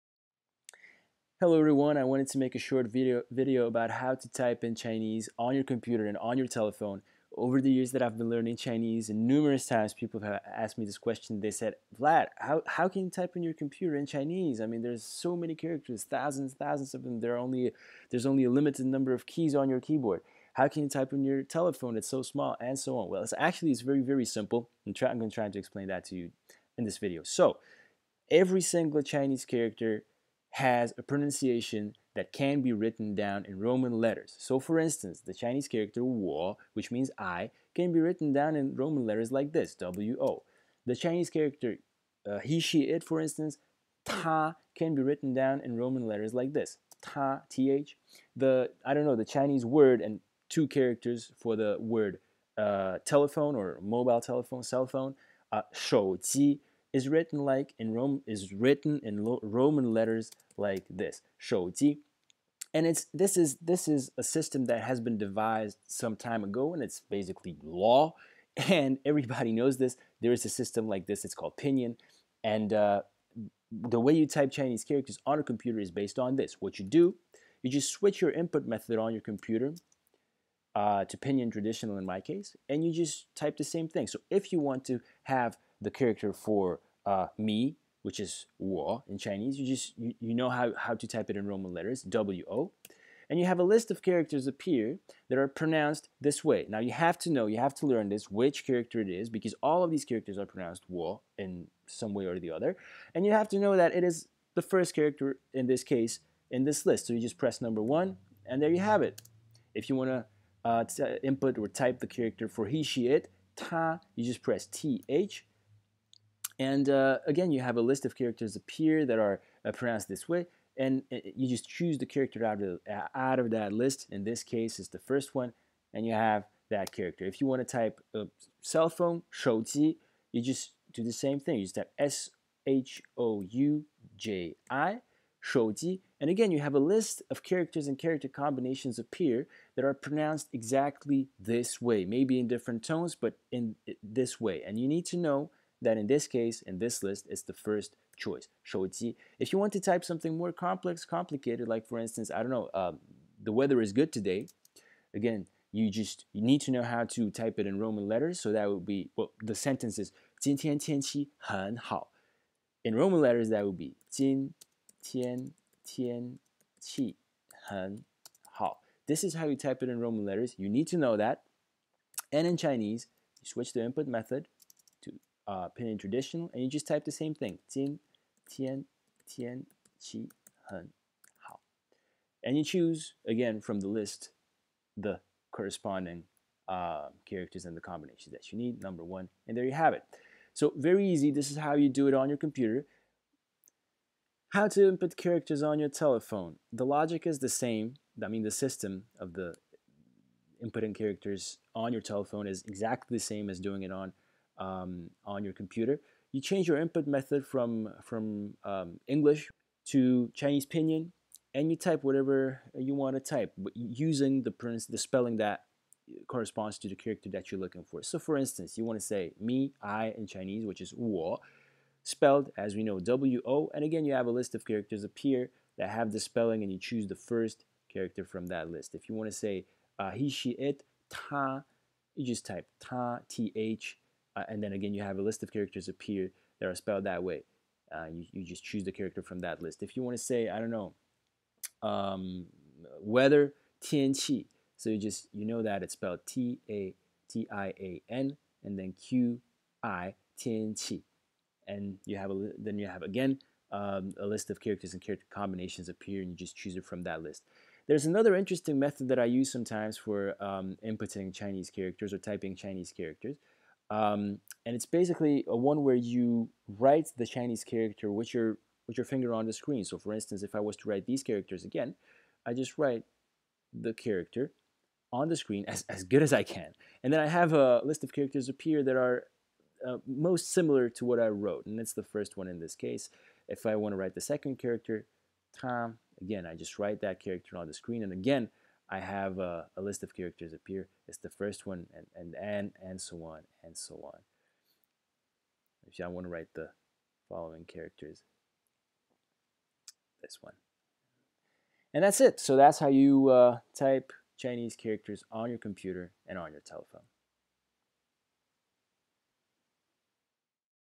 How to type in Chinese
A video explaining how to input Chinese characters on your computer and phone.

How to type in Chinese? What is pinyin? How to use it? I tried to answer these questions in the video.